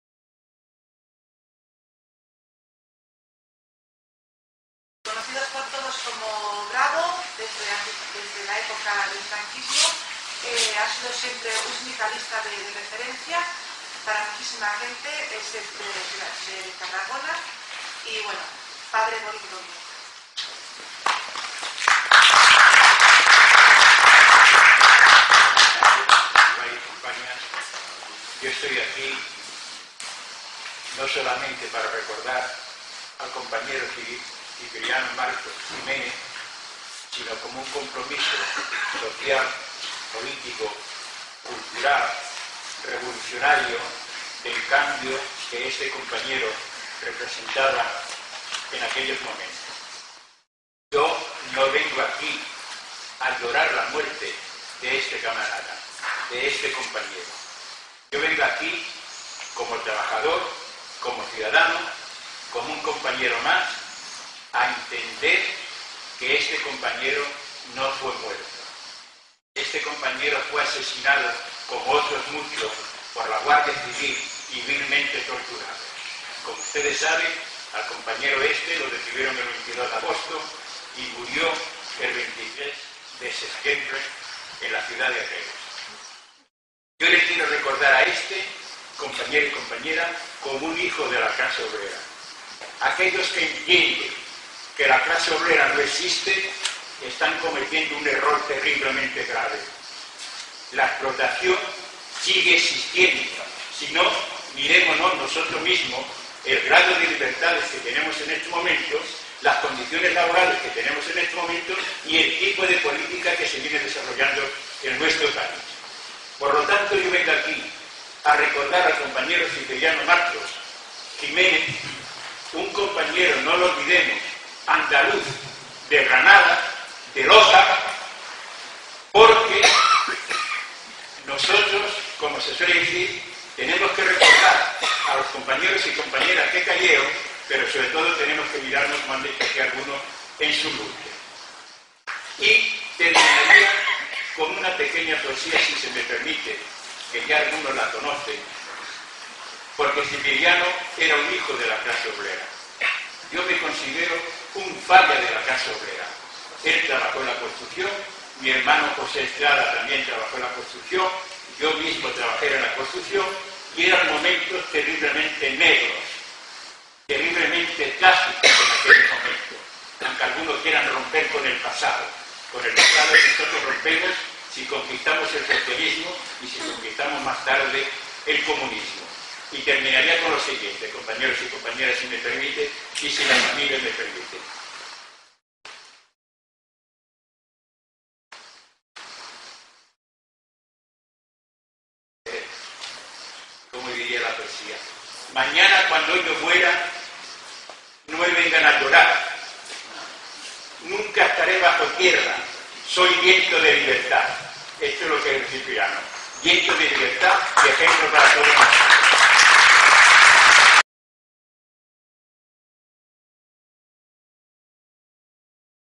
Martin Bravo. Homenatge a Cipriano Martos Jiménez al cemintiri de Reus.
Per Dignitat. Ni oblidem ni perdonem !!!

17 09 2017